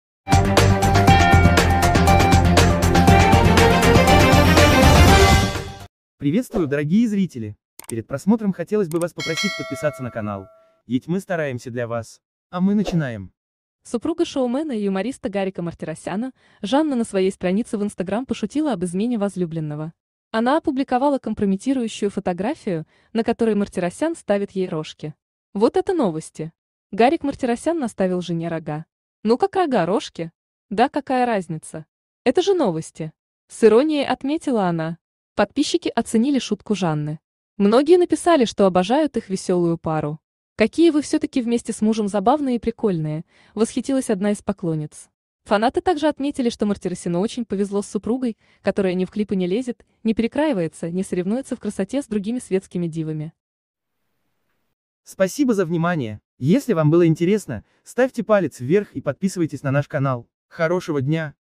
«Наставил рога»: жена пошутила об «измене» Мартиросяна
Супруга шоумена и юмориста Гарика Мартиросяна Жанна на своей странице в Instagram пошутила об «измене» возлюбленного.